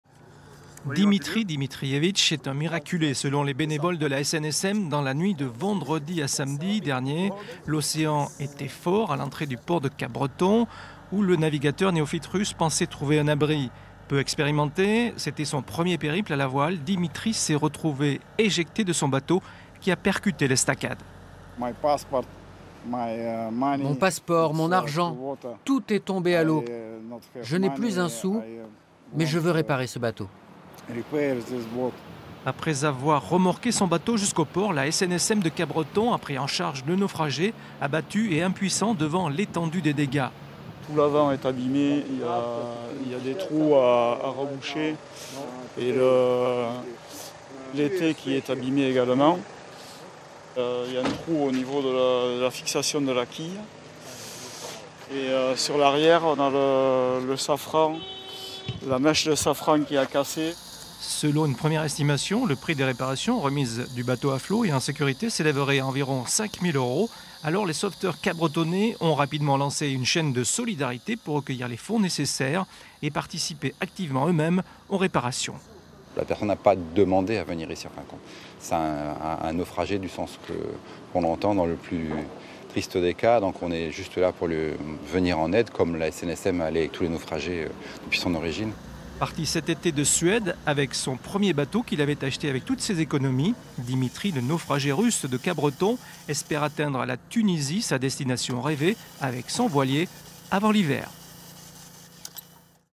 Un navigateur néophyte russe en difficulté après son naufrage à Capbreton (Landes)
Intervenants : Dimitri Dmitrievitch, Naufragé, Dominique Contis, Patron bateau SNSM Capbreton, Patrick Muzellec, Président SNSM Capbreton

Equipe : PANNIER Patrick, MONTIEL Laurent, LAURENT D.